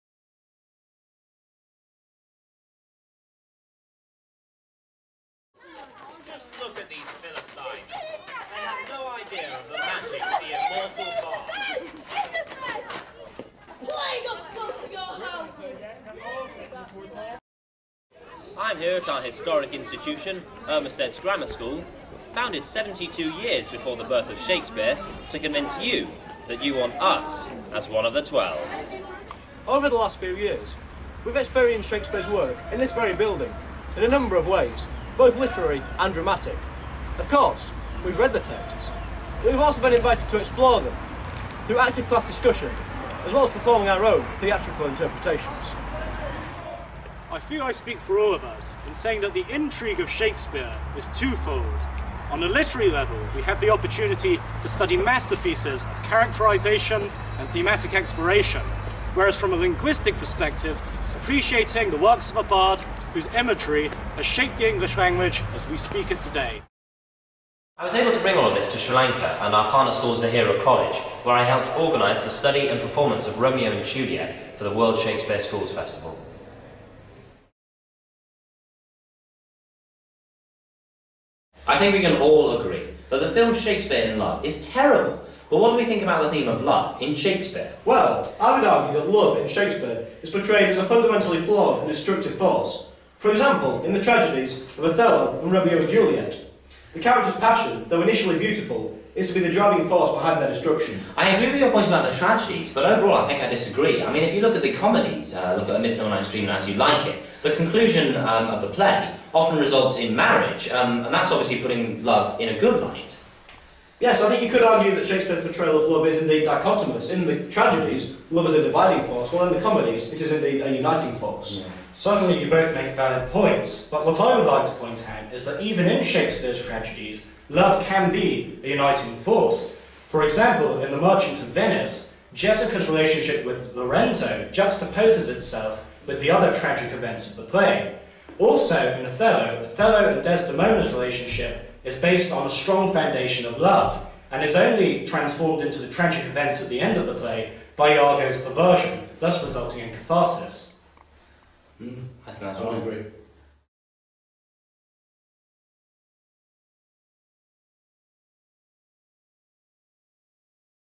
Ermysted's Grammar School - The Great Shakespeare Debate 2009/2010
Ermysted's Grammar School - Video entry for the Great Shakespeare Debate 2009/2010.

Theme: Love